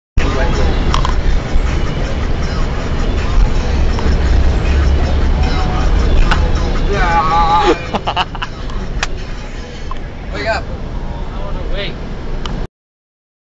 Wet Willy
Andrew givin gary a wet willy after he fell asleep. check out the intensity of andrews face when he starts, then when things go south, he just resorts to slappin gary :-P